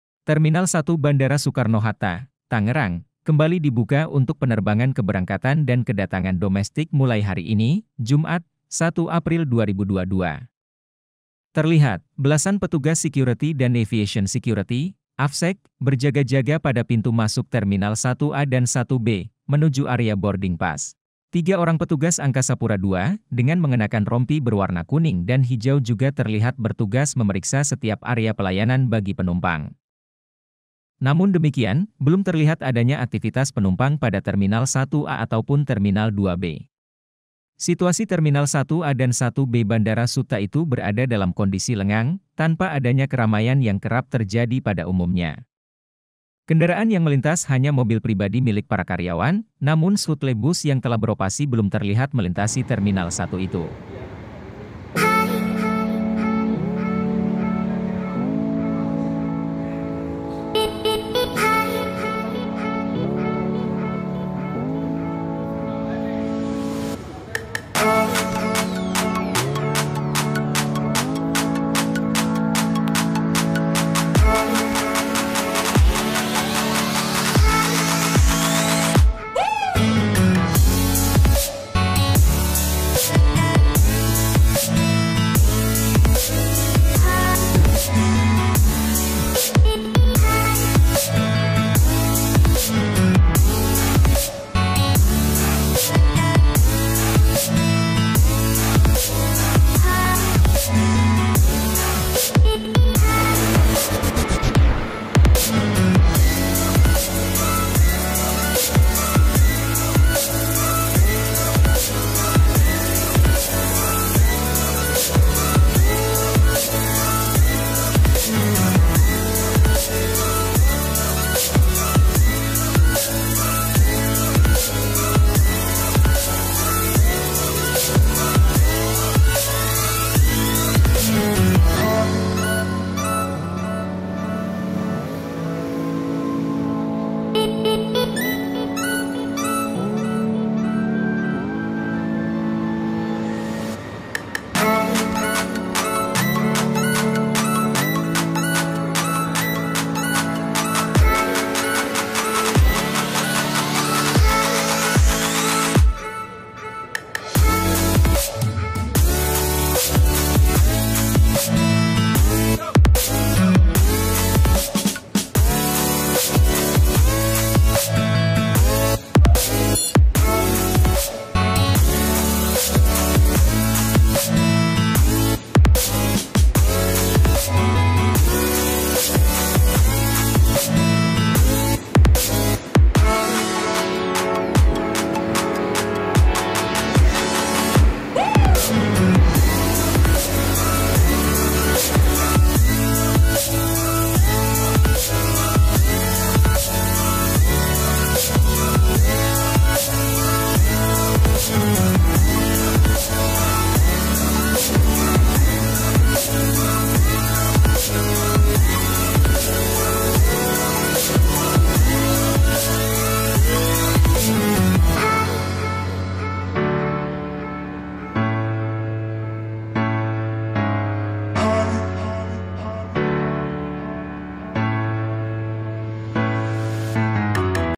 Hari Pertama Kembali Dibuka, Begini Kondisi Terminal 1 Bandara Soekarno-Hatta
WARTAKOTALIVE.COM, Tangerang - Terminal 1 Bandara Soekarno-Hatta, Tangerang, kembali dibuka untuk penerbangan keberangkatan dan kedatangan domestik mulai hari ini, Jumat (1/4/2022).

Melalui pantauan Wartakotalive.com, terminal yang kembali dioperasikan ialah terminal 1A dan 1B Bandara Soetta.

Terlihat, belasan petugas security dan Aviation Security (Avsec) berjaga-jaga pada pintu masuk terminal 1A dan 1B, menuju area boarding pass.

Tiga orang petugas Angkasa Pura II, dengan mengenakan rompi berwarna kuning dan hijau juga terlihat bertugas memeriksa setiap area pelayanan bagi penumpang.

Namun demikian, belum terlihat adanya aktivitas penumpang pada terminal 1A ataupun terminal 2B.

Situasi Terminal 1A dan 1B bandara Soetta itu berada dalam kondisi lengang, tanpa adanya keramaian yang kerap terjadi pada umumnya.

Video Jurnalis  :  Gilbert Sem Sandro